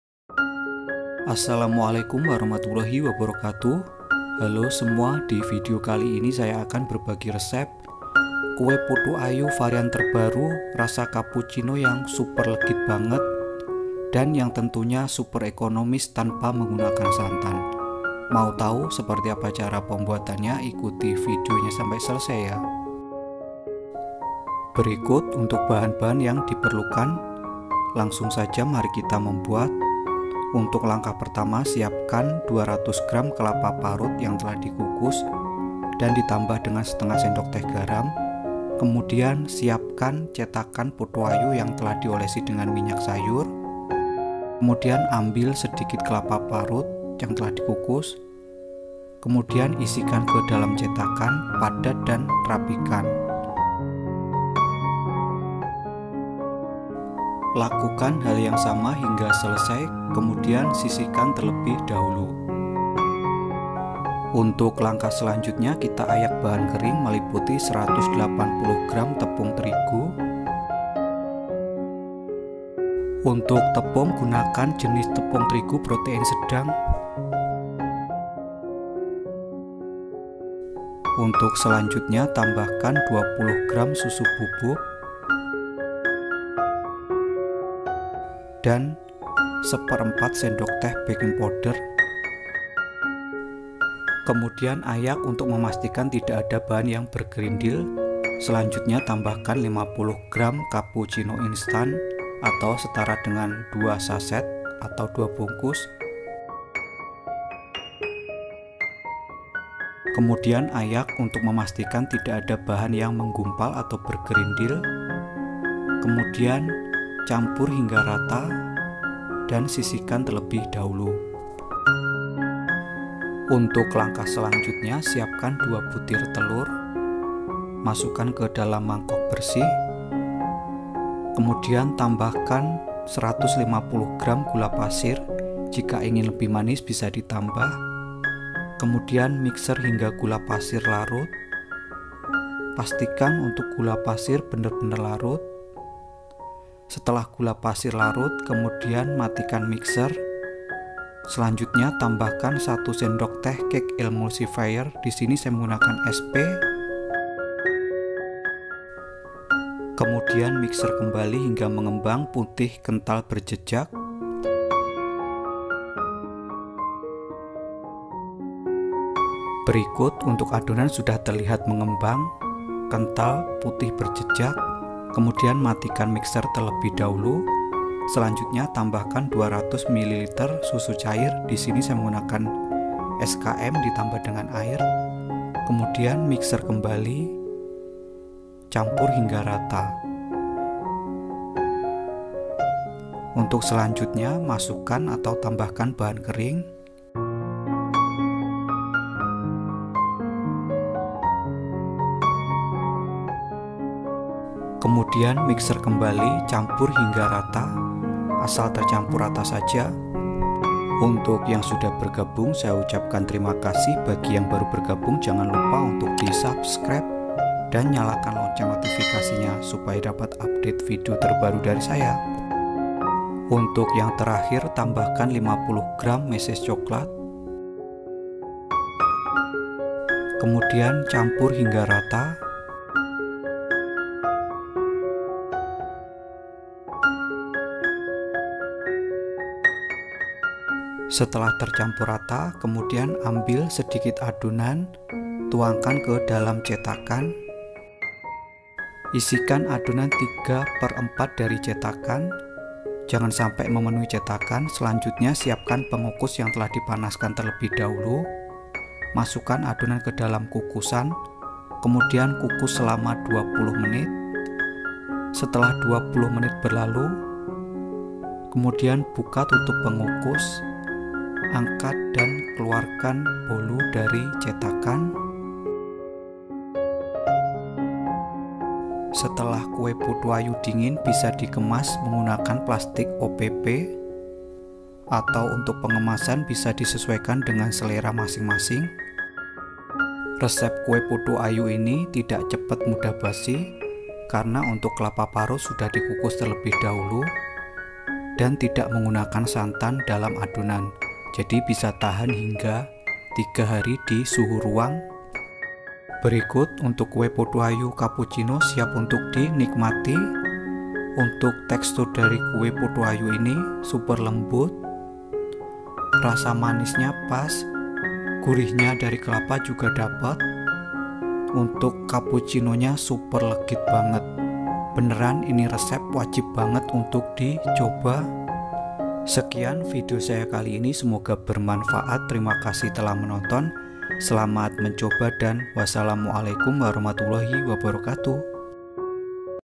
RASAKAN SENSASI CAPPUCINO DALAM BOLU || KUE PUTU AYU VARIAN RASA TERBARU
RASAKAN SENSASI CAPPUCINO DALAM BOLU NYA LEGIT BANGET

Pengen bolu dengan rasa terbaru, berikut solusinya. Kue Putu Ayu Cappucino yang super legit banget. bahannya pun cukup ekonomis dan mudah didapat. selain sebagai camilan kue ini cocok dijadikan sebagai ide jualan dan ide isian snack box. yuk cobain resepnya :

-200 gram kelapa parut
-1/2 sdt garam
-2 butir telur
-150 gram gula pasir
-1 sdt cake emulsifier
-180 gram tepung terigu
-20 gram susu bubuk
-50 gram kapucino instan
-1/2 sdt baking powder
-200 ml susu cair ( 2 bungkus SMK ditambah air hingga 200 ml)

Untuk Caranya silahkan ditonton Videonya sampai selesai.

Selamat Mencoba...

Tonton juga Video saya sebelumnya

*English*

FEEL THE CAPPUCINO SENSATION IN THE BOLU IT'S VERY LEGIT

If you want a sponge with the latest flavor, here's the solution. Putu Ayu Cappucino cake which is super legit. the material is also quite economical and easy to obtain. Apart from being a snack, this cake is suitable as a selling idea and idea for stuffing a snack box. let's try the recipe:

-200 grams of grated coconut
-1/2 tsp salt
-2 eggs
-150 grams of sugar
-1 tsp cake emulsifier
-180 grams of wheat flour
-20 grams of powdered milk
-50 grams of instant cappuccino
-1/2 tsp baking powder
-200 ml of liquid milk (2 packs of SMK plus water up to 200 ml)

For the method, please watch the video until it's finished.

Good luck...

Also watch my previous video